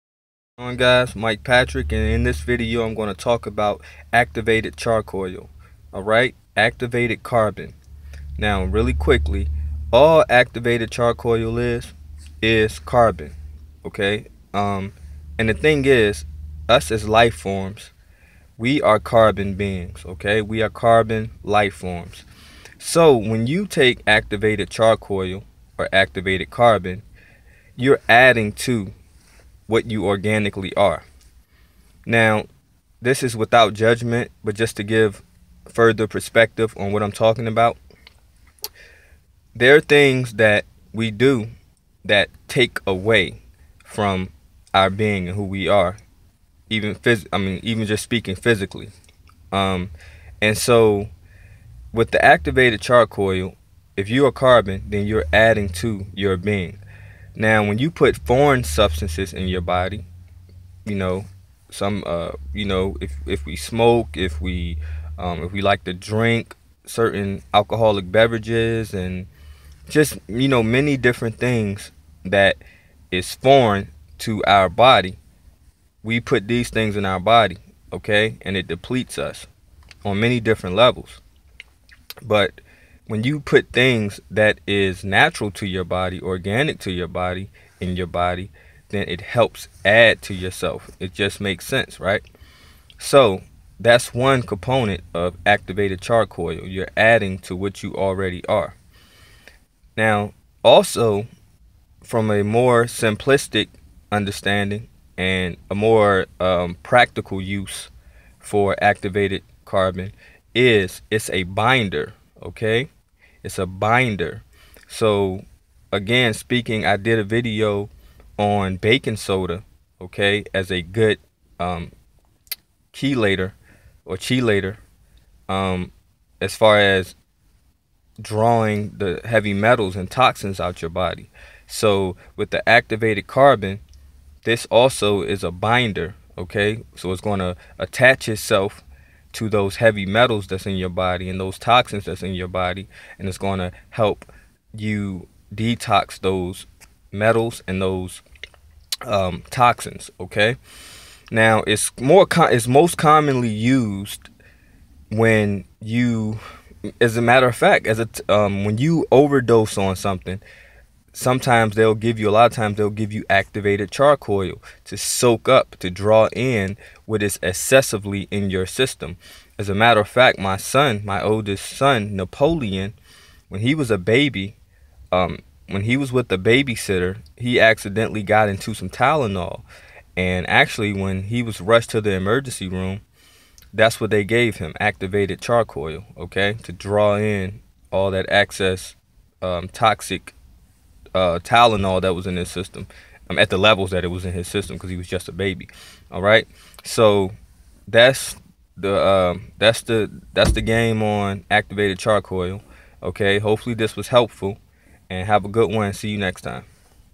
Activated charcoal. Activated carbon.Detox, binder, heavy metals, toxin chelation.
In this video I go over some beneficial uses for activated charcoal